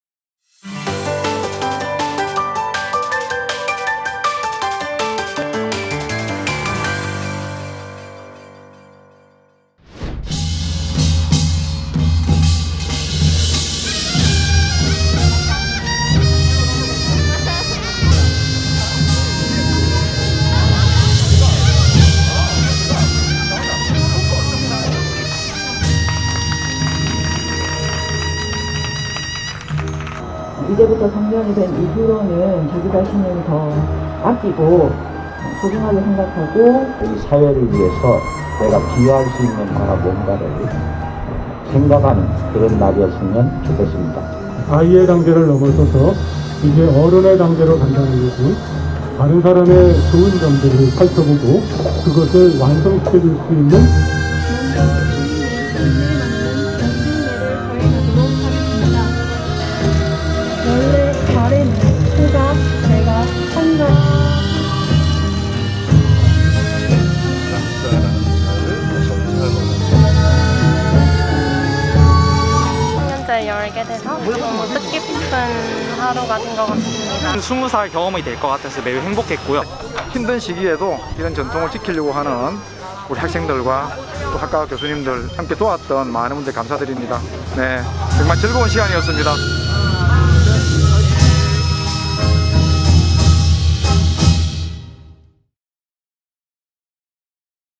[PNU뉴스]부산대 「2021 효원성년제」
부산대, 전통 성년식 「2021 효원성년제」 개최
‘위드 코로나’ 대학 캠퍼스가 먼저 기지개 켭니다~!
- 11월 3일 오후 인문대 교수연구동 야외공간에서 행사…스무 살 부산대 학생들 전통의례 참가
- 부산대 효원성년제는 1999년에 시작해 올해 23회째 진행되고 있는 전통성년의식